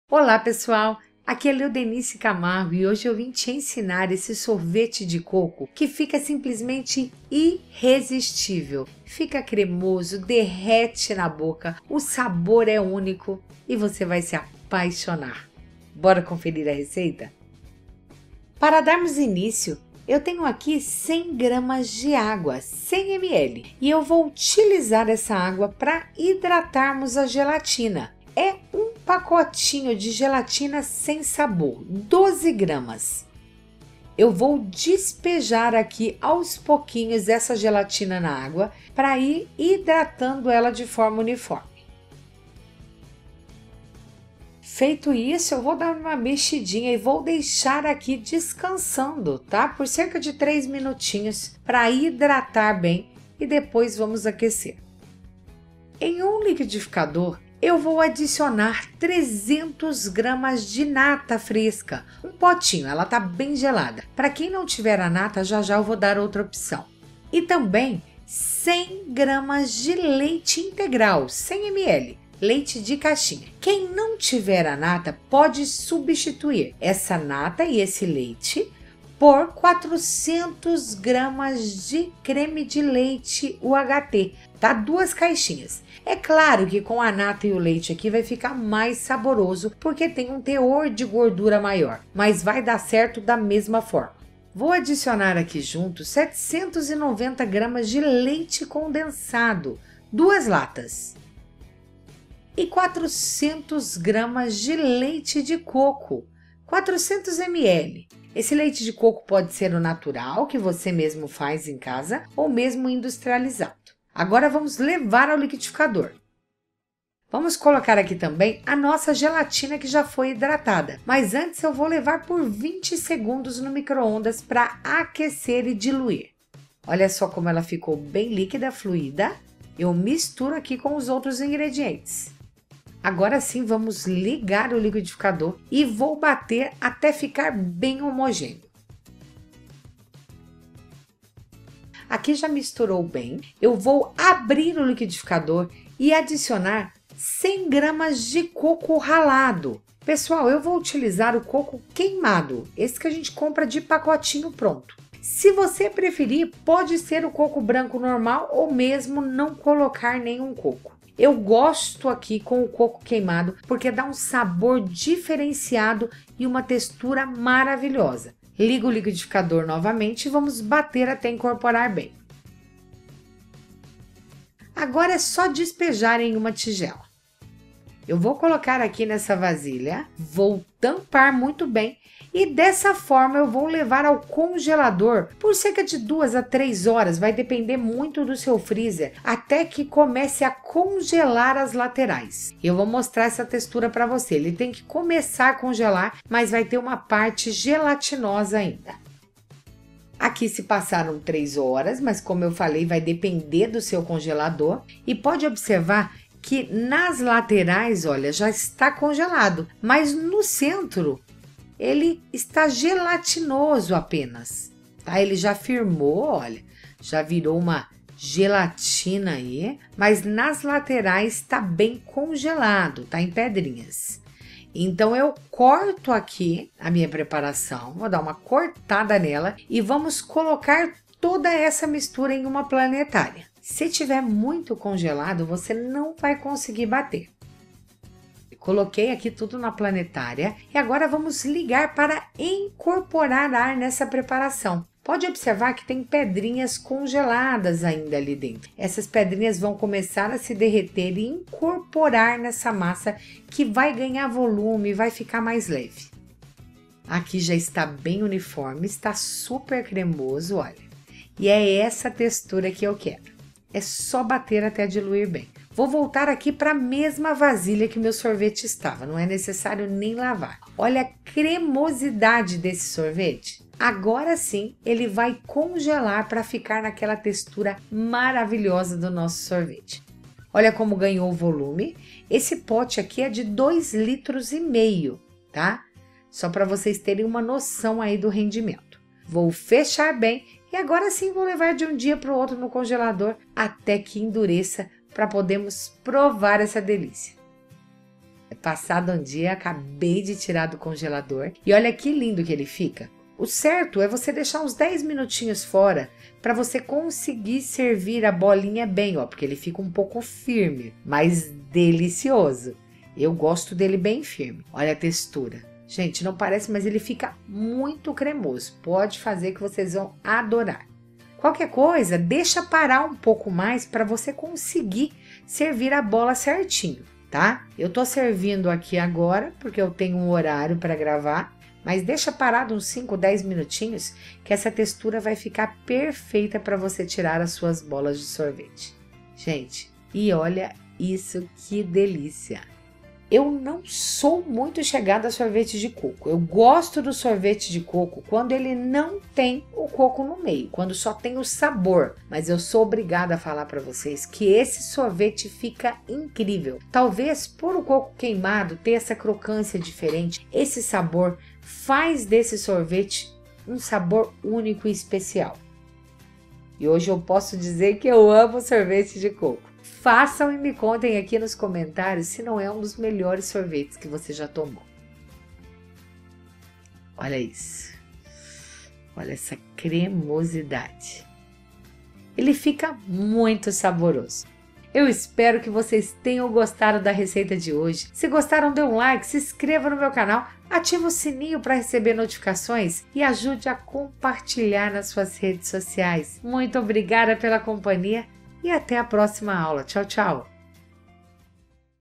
SORVETE DE COCO CASEIRO 🥥 COM INGREDIENTES SIMPLES
Nada melhor nesse calor que esse delicioso Sorvete de Coco , feito no liquidificador e com ingredientes simples, que todos tem em casa, bora conferir essa delicia !!! 😉💜

✔INGREDIENTES:
- 790g de Leite Condensado (2 Latas)
- 400g de Leite de Coco (2 Vidrinhos)
- 300g de Nata (1 Pote)
- 100g de Coco Ralada Queimado (1 Pacotinho)
- 100g de Água (100ml)
- 100g de Leite Integral (100ml)
- 12g de Gelatina sem Sabor (1 Envelope)

RENDIMENTO: 2,5 Litros.